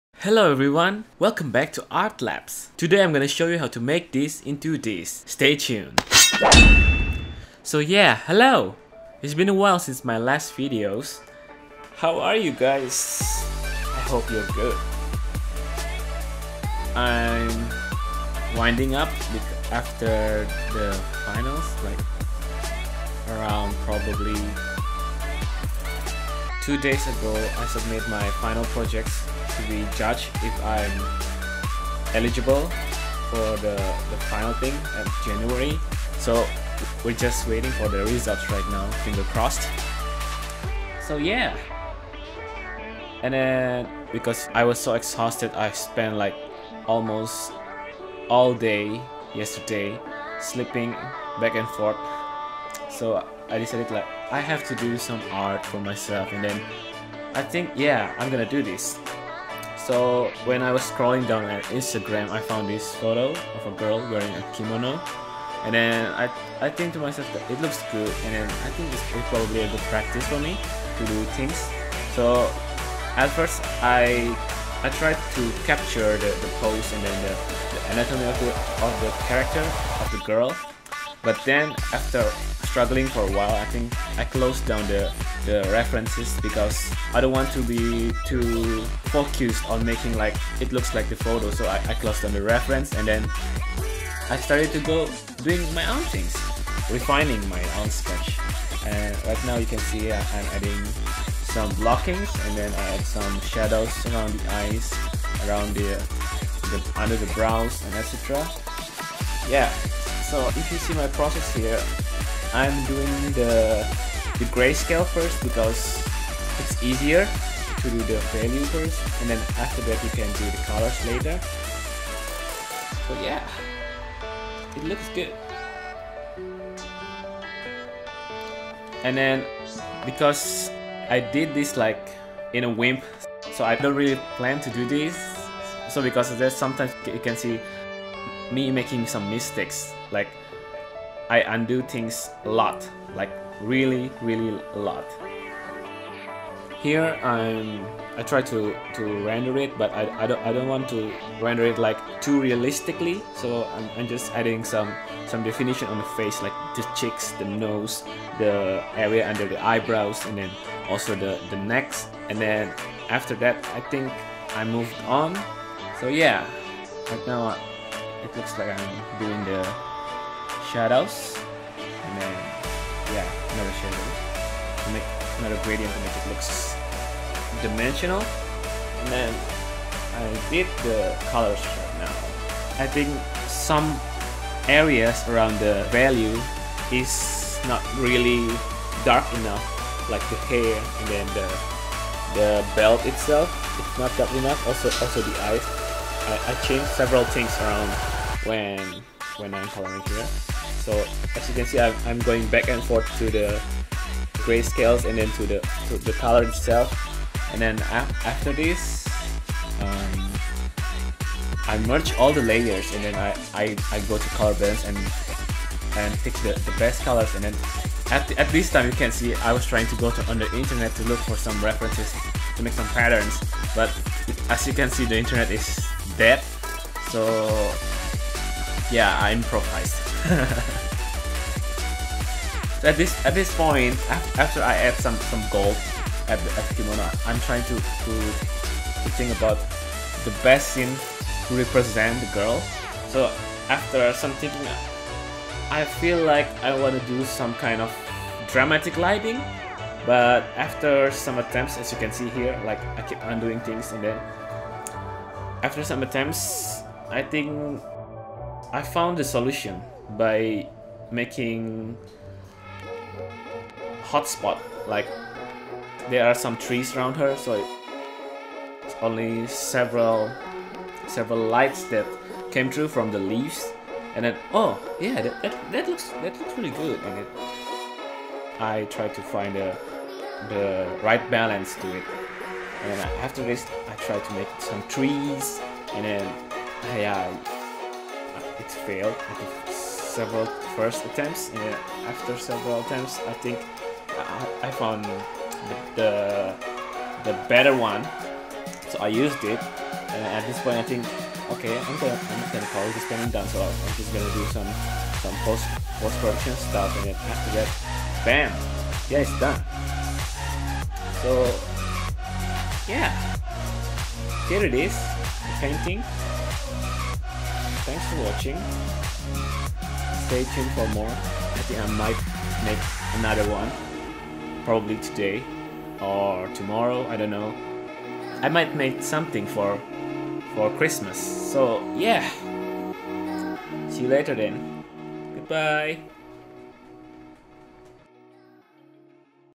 ARTLAPSE - Kimono Girl
Sorry for the rambling, still not used to doing voice over XD.

Refference from : @hmassy at instagram